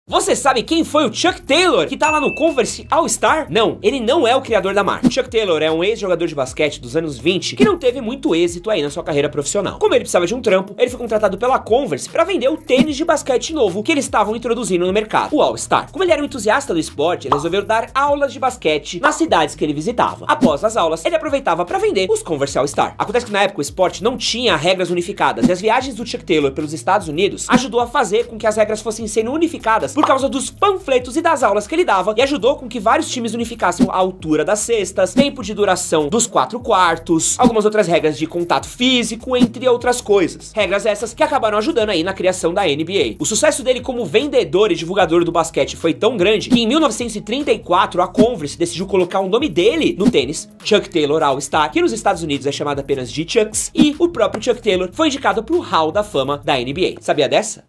VOCÊ CONHECE A HISTÓRIA DO CHUCK TAYLOR ALL STAR?
Você conhece a história do Chuck Taylor do Converse All Star? No de hoje, Edson Castro nos conta a história de Chuck Taylor e a importância dele no All Star.